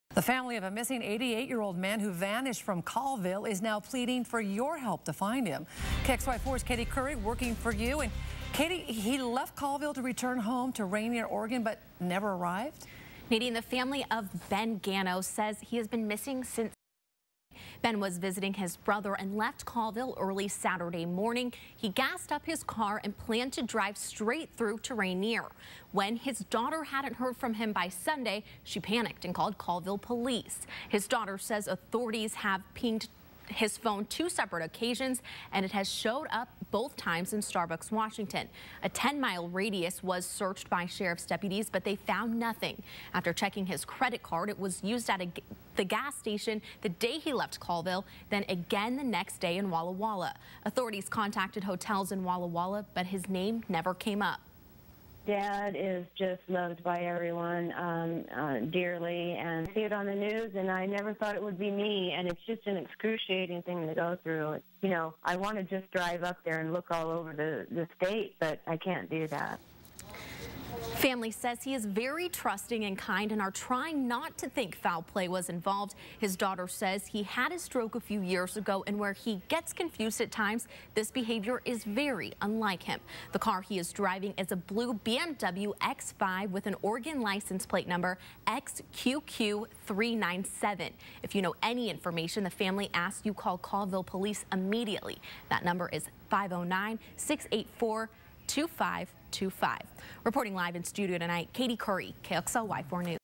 Man disappears on trip from Colville to Oregon
The family of a missing 88-year-old Oregon man who vanished from  Colville after visiting his brother is making a plea for the public's  help to find him. KXLY4's Caiti Currey reports.